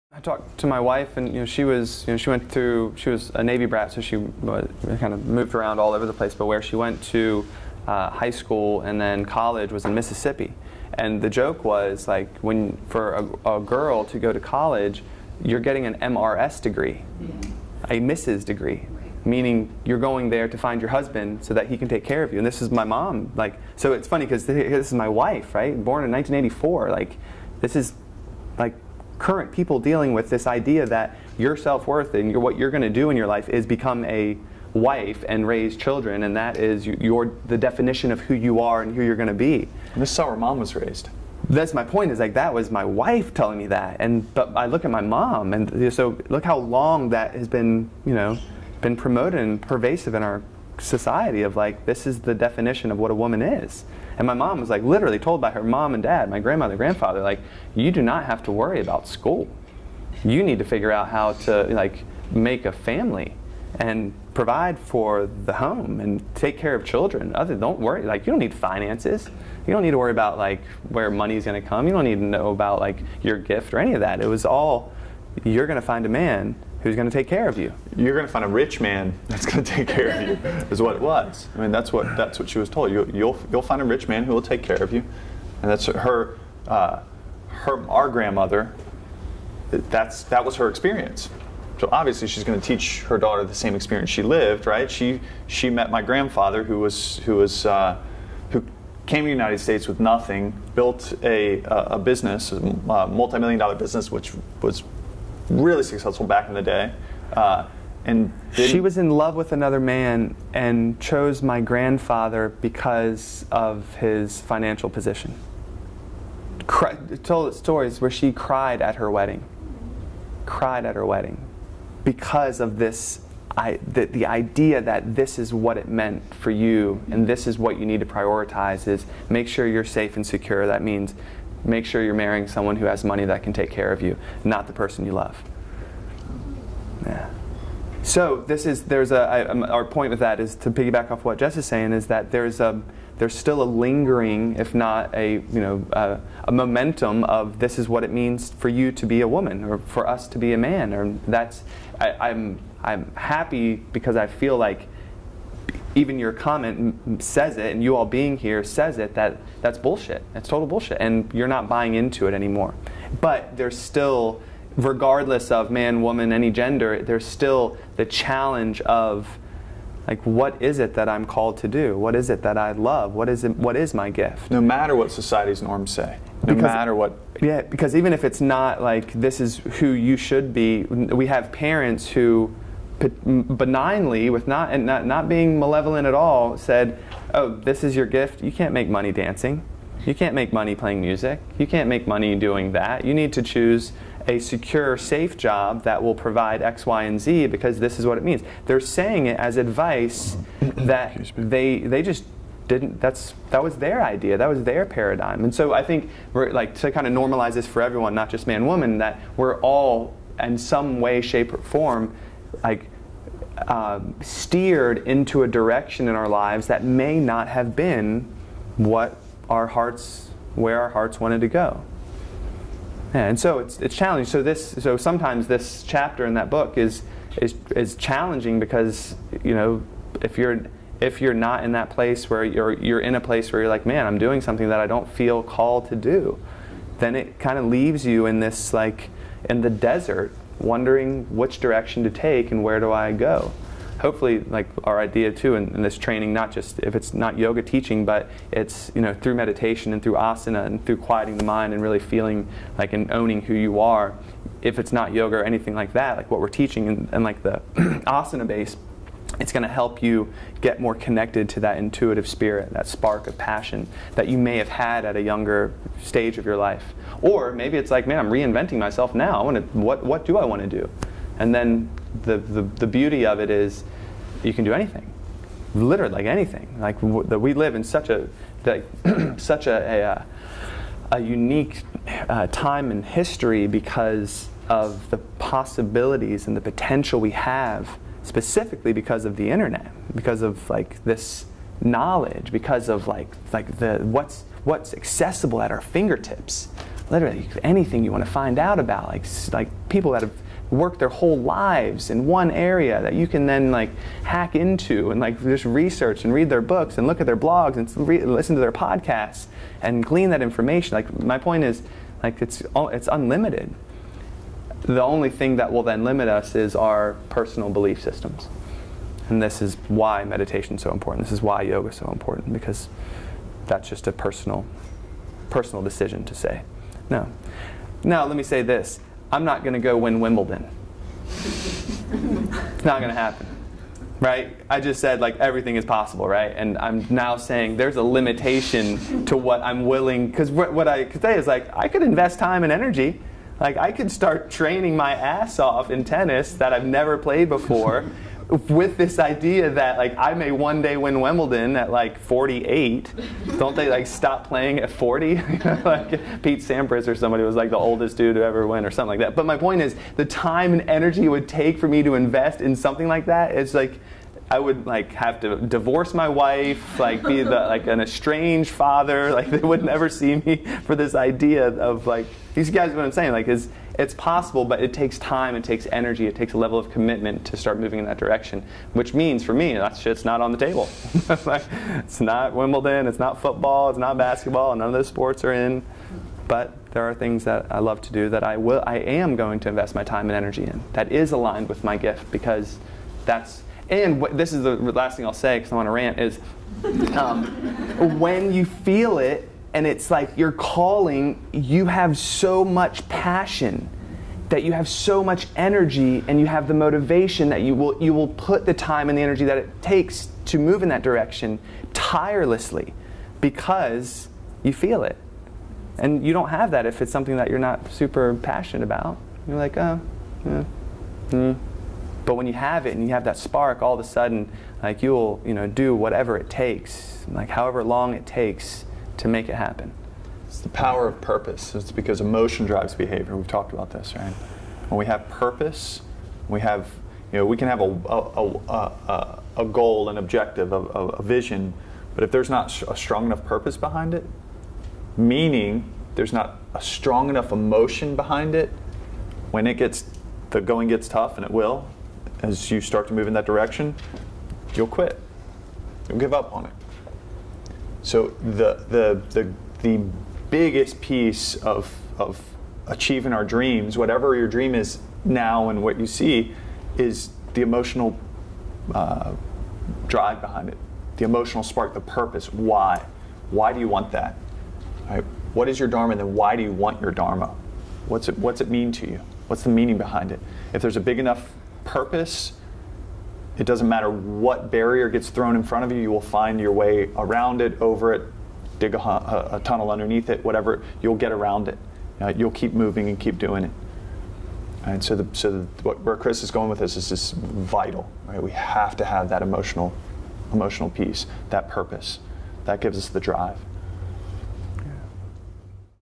The Gift You Don't Know You Have
From their Teacher Training Video Vault, John and Chris Yax, co-founders of Yax Yoga Concepts and Hot House Yoga, explain the concept of Finding Your Gift.

Chris describes what he learned from his wife about the culture of the deep South in regard to what going to college really meant for many women, and then relates it to how similar beliefs were taught to his mother by her parents as she was raised.

These thoughts lead into the discussion of how you can do or become anything you want no matter who you are and the challenges of finding the path that is right for you; the direction you want you’re life to take.

He talks about how in our present world anything is possible. To do or become anything is within reach due to this era of time in which we live – the age of the internet.

“We live in such a unique time in history because of the possibilities and the potential we have, specifically because of the internet; because of this knowledge; because of what’s accessible at our fingertips. Literally anything you want to find out about, people who have worked their whole lives in one area that you can then hack into, and just research, read their books, and look at their blogs, listen to their podcasts, and glean that information. My point is it’s unlimited.”

Chris goes on to say that, “The only thing then that will limit us is our personal belief systems. This is why meditation is so important. This is why yoga is so important because that’s just a personal decision to say no.”

Of course, he clarifies his thoughts on “anything is possible” by illustrating that there are other limitations of what you may be willing to do. For example, He says, “I’m not going to go win Wimbledon. Not going to happen, right.”

There is a level of commitment that is required for anything you decide to pursue.  For Chris, he is not willing to invest what is needed for him to win Wimbledon.

The main thing that both John and Chris express is whatever it is that you determine to be your calling, your gift, you have a tremendous amount of  passion for it because it’s something you love. Along with that passion you have a great deal of energy and the motivation that will drive you to invest whatever amount of time and energy that is required in order for you to reach your goal.

John explains, “It’s the power of purpose that’s because emotion drives behavior.”  There must be a strong enough purpose and emotion behind what you’re trying to achieve because if you don’t have that, when things begin to get hard you will give up on it.

“If there’s a big enough purpose, it doesn’t matter what barrier gets thrown in front of you, you will find your way around it, over it, dig a tunnel underneath it, whatever, you’ll get around it. You’ll keep moving. You’ll keep doing it.”